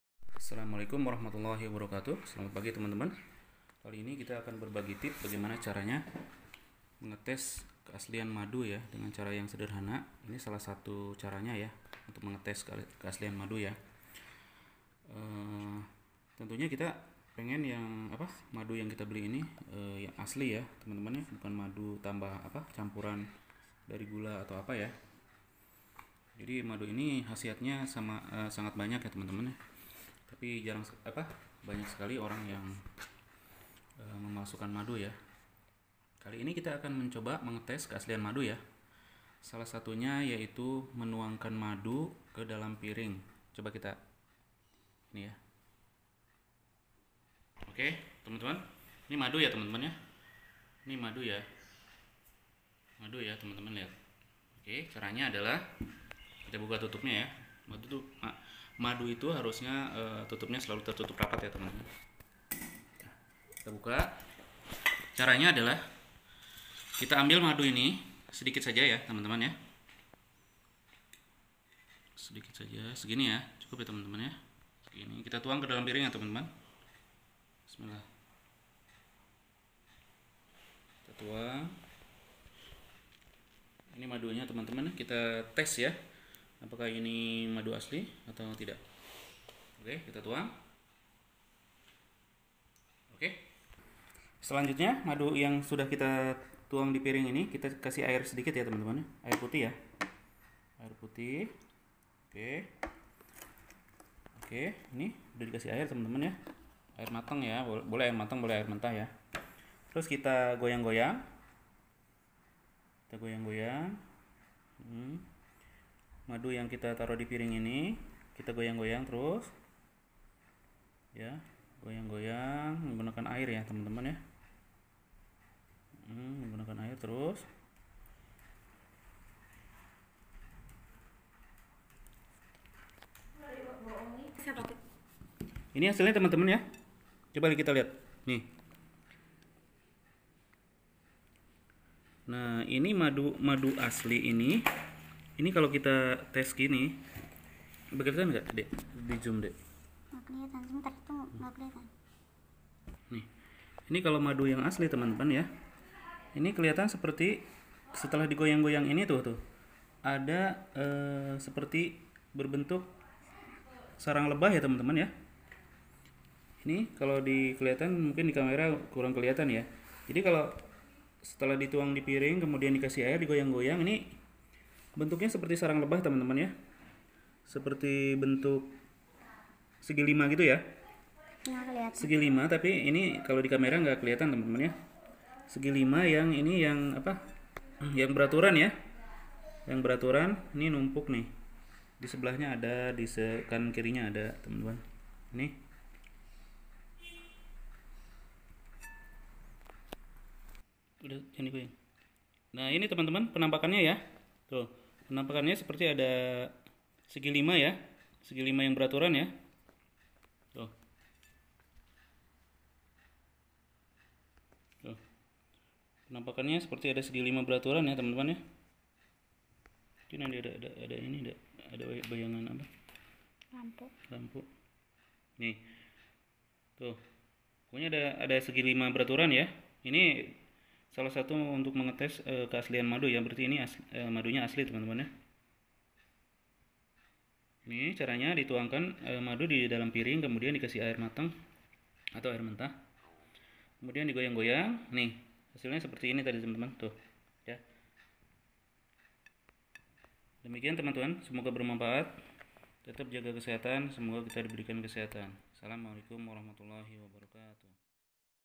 CARA MENGETES MADU TERNYATA AJAIB | Ngak bisa dibohongin ASLI/ PALSU
iniPada video ini saya ingin berbagi tip CARA MENGETES MADU ASLI/ PALSU. Caranya sangat mudah sekali.

Silahkan tonton videonya ya...